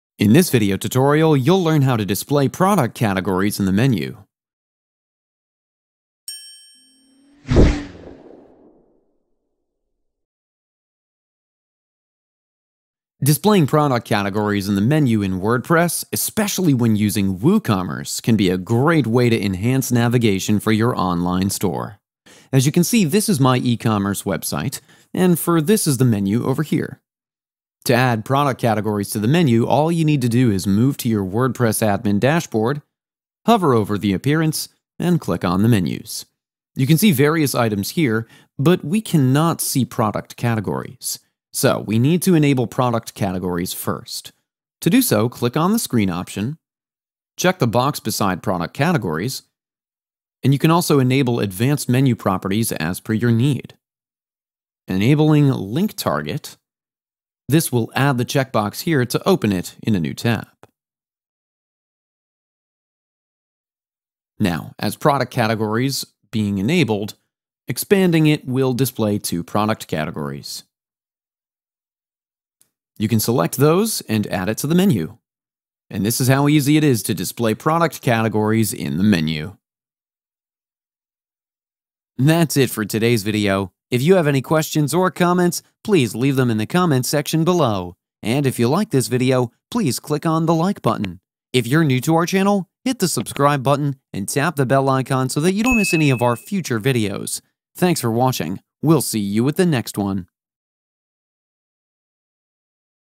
How To Display Product Categories In Menu In WordPress
This video is about how to Display Product Categories In Menu In WordPress. Displaying product categories in the menu in WordPress, especially when using WooCommerce, can be a great way to enhance navigation for your online store.

0:00 - Introduction
0:30 - Navigate to Menu
0:43 - Enable Product Categories Option